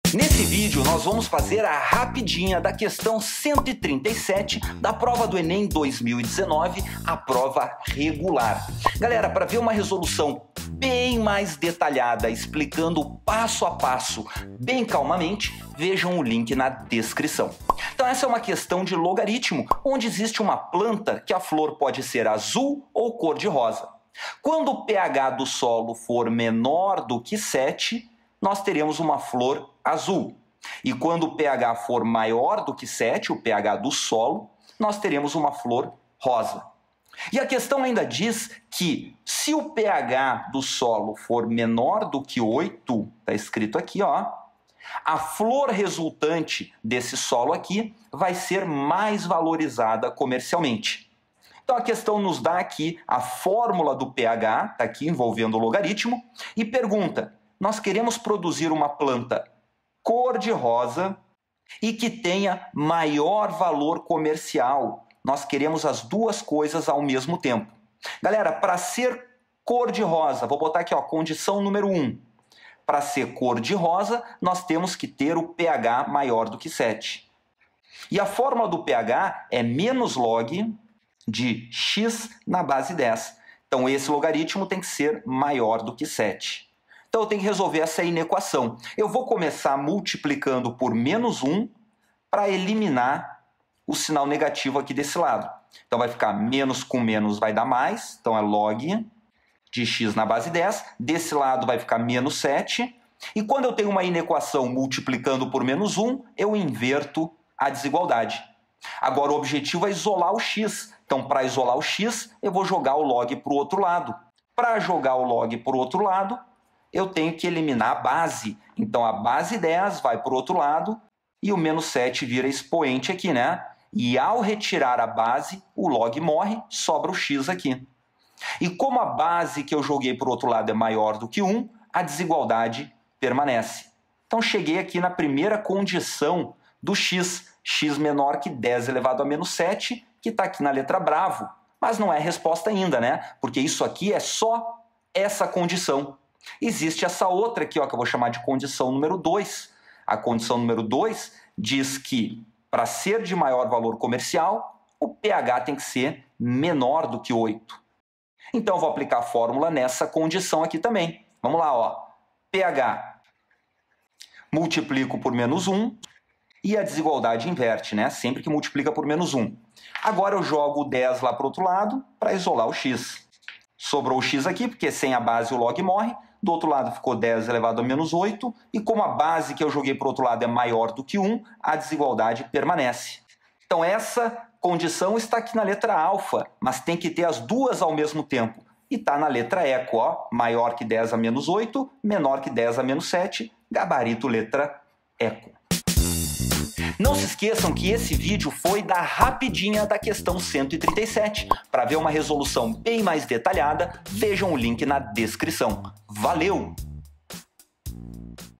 🏃[ENEM 2019] 137 📘 LOGARITMO A Hydrangea macrophylla é uma planta com flor azul ou cor-de-rosa,
A Hydrangea macrophylla é uma planta com flor azul ou cor-de-rosa, dependendo do pH do solo no qual está plantada. Em solo ácido (ou seja, com pH 7) a flor é azul, enquanto que em solo alcalino (ou seja, com pH 7) a flor é rosa. Considere que a Hydrangea cor-de-rosa mais valorizada comercialmente numa determinada região seja aquela produzida em solo com pH inferior a 8. Sabe-se que pH = - log10x, em que x é a concentração de íon hidrogênio (H+).

ENEM 2019 - 1ª Aplicação - Prova de Matemática Resolvida pelo Prof. Caju.

Problema sobre Logaritmo

Questão 136 da Prova Azul - ENEM 2019

Quer contribuir para manter meu trabalho de compartilhar conhecimento pra todo Brasil?